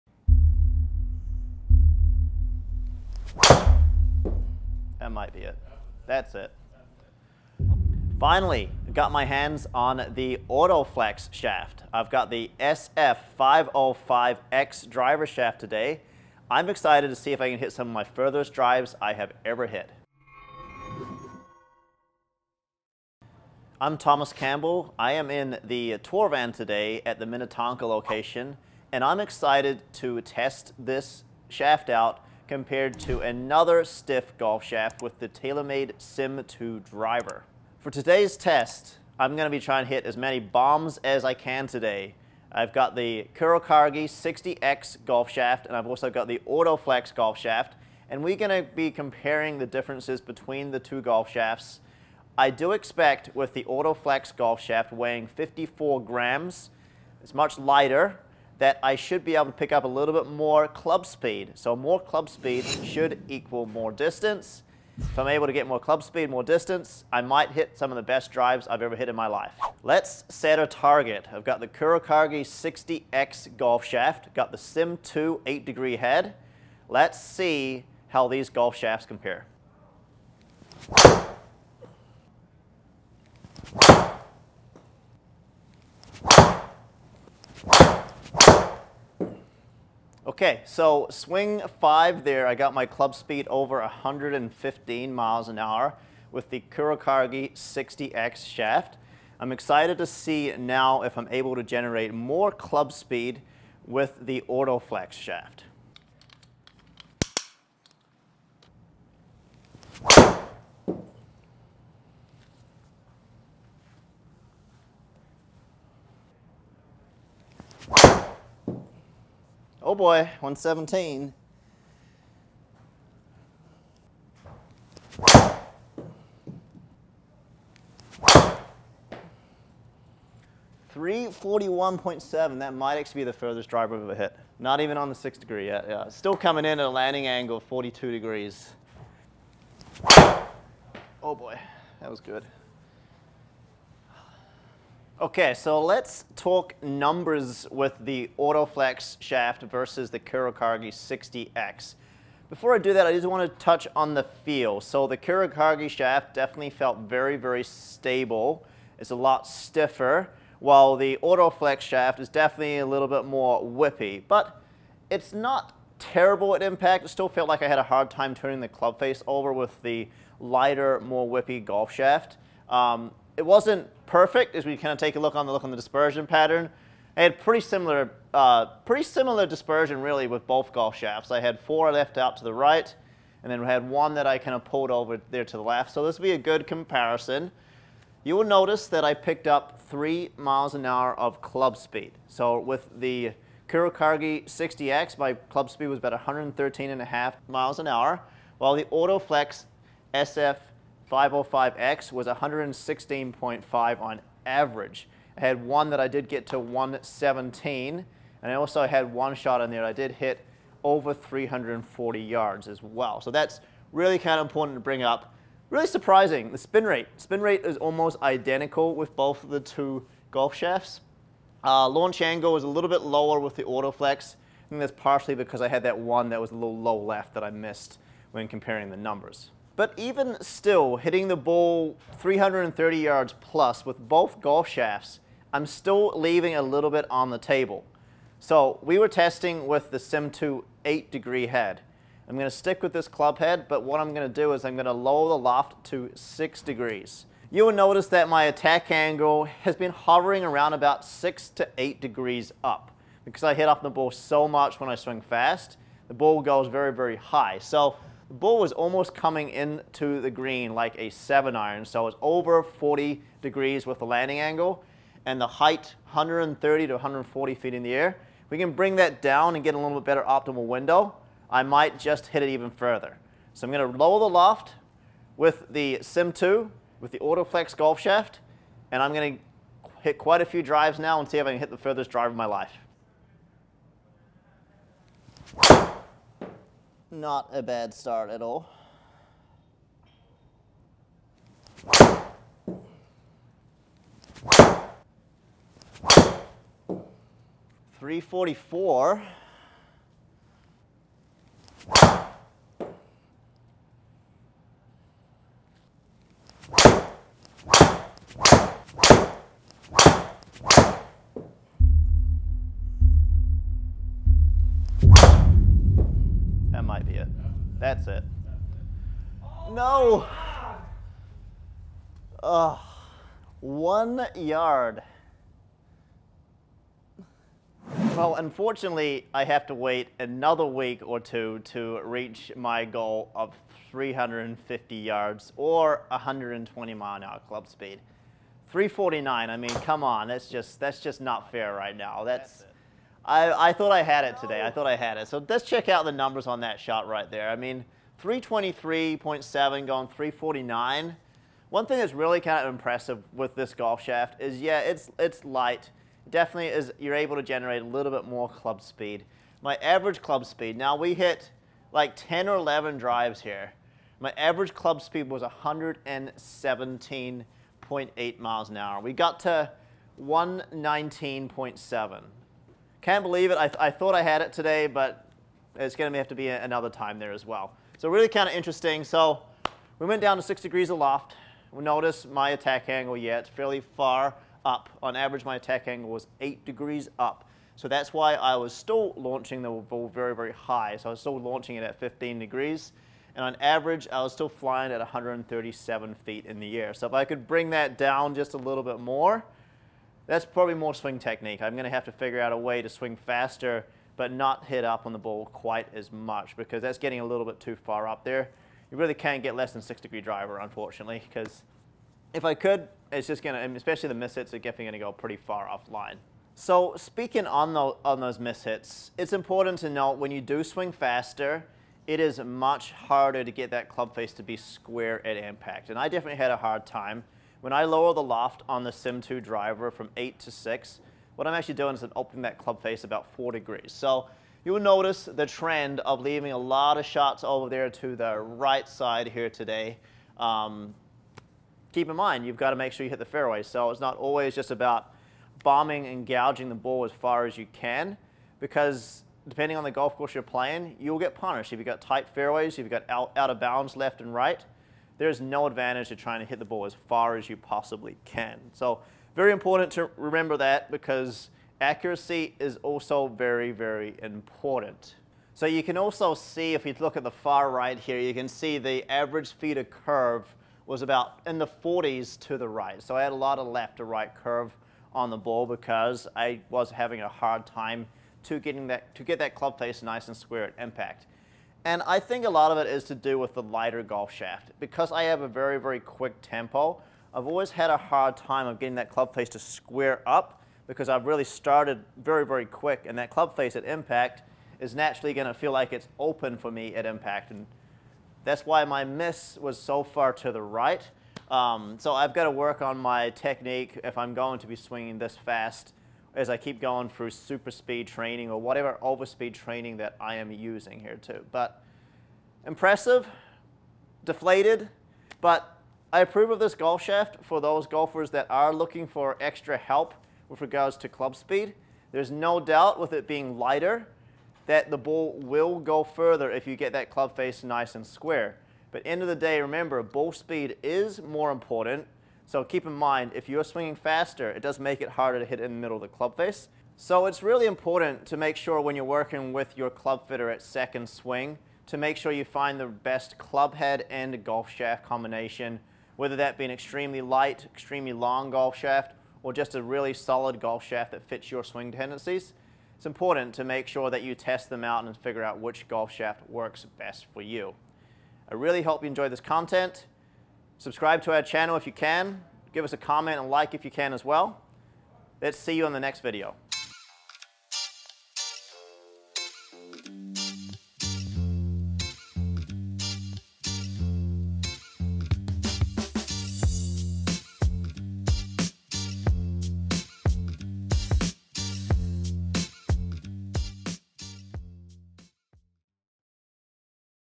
AutoFlex Golf Shaft Review | Furthest Drive & Fastest Club Speed Ever
The AutoFlex golf shaft is quickly gaining popularity due to its unique construction and properties that enable easier and faster swinging. 2nd Swing master fitter Thomas Campbell tests the AutoFlex golf shaft with the help of Trackman and registers his highest club speed ever.